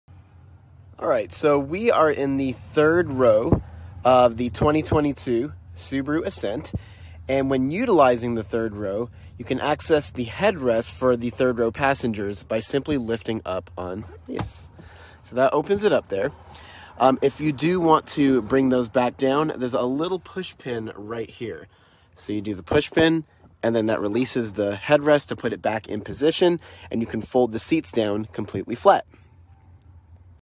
3rd Row Headrests in 2022 Subaru Ascent
Steven shows viewers how to lower and raise the third row seat headrests in the 2022 Subaru Ascent.

Have another question? Please ask it in the comment section and we will create a video for you.